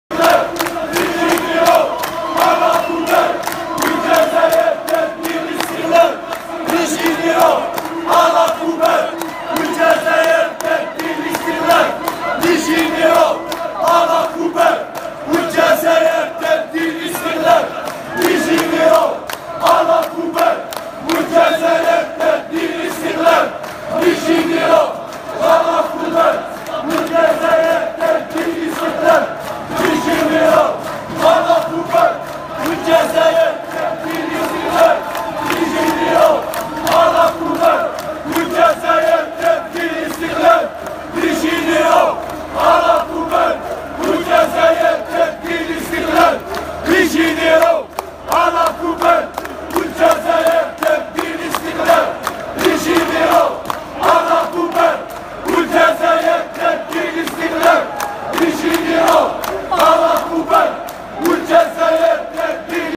إطلاق سراح صهيب دباغي و وضعه تحت الرقابة القضائية Soheib Debbaghi mis sous contrôle judiciaire
Le hirakiste Soheib Debbaghi ancien détenu d'Opinion relâché et mis sous contrôle judiciaire.
الحراكي صهيب دباغي معتقل الرأي سابقا يطلق سراحه من شرطة شاطونوف بابعاصمة و ووضعه تحت الرقابة القضائية.